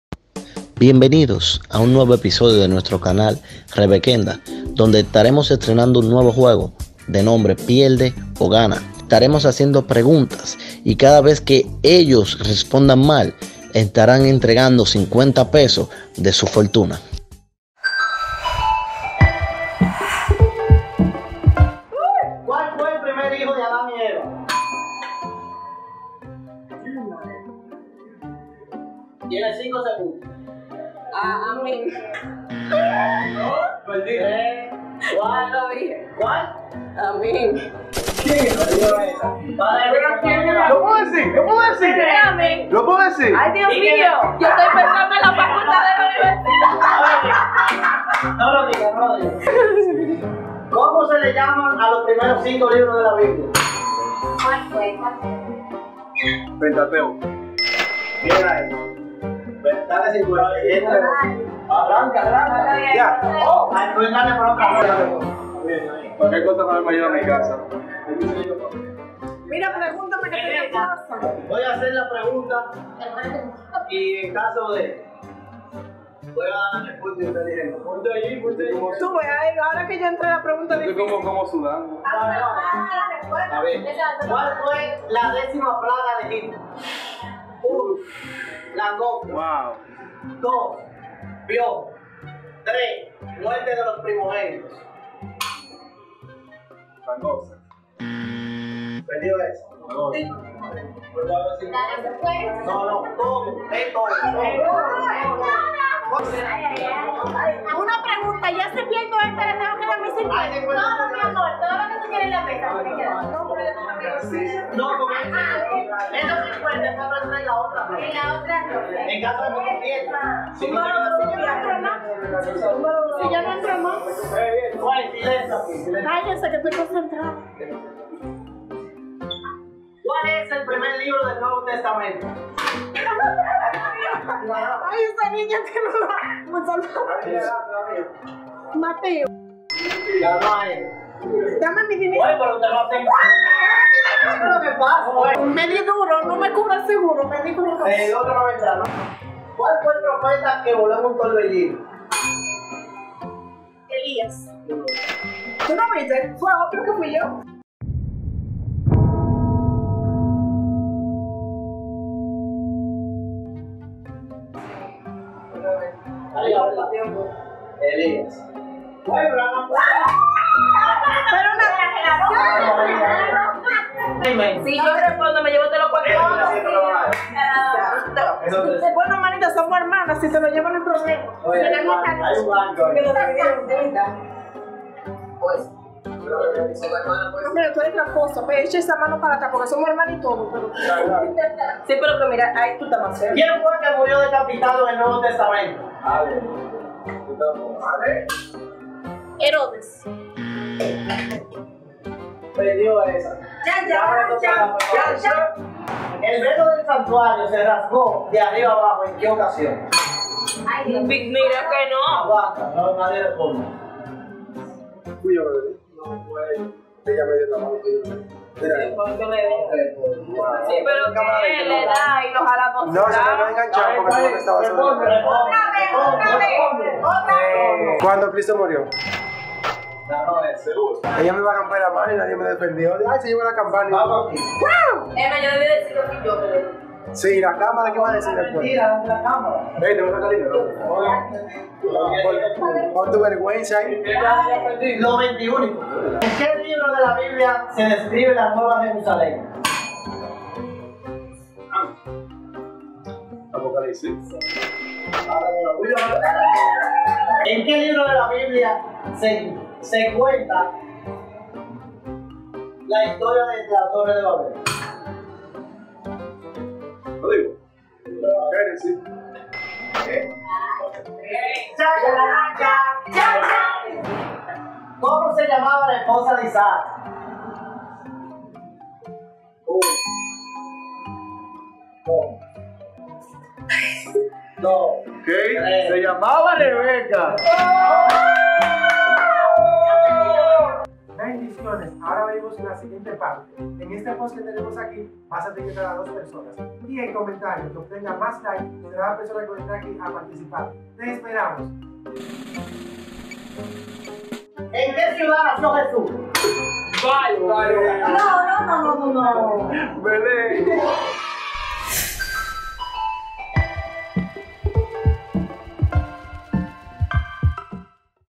Revequenda - PIERDE O GANA - Debate Biblia 001 - T0
Bienvenidos a "REVEQUENDA" el único canal cristiano en youtube  donde puedes aprender y divertirte al mismo tiempo.

En esta ocasión te presentamos  el primer episodio de "Pierde o Gana" un juego en el que solo puede haber un solo ganador

►SIGUEME!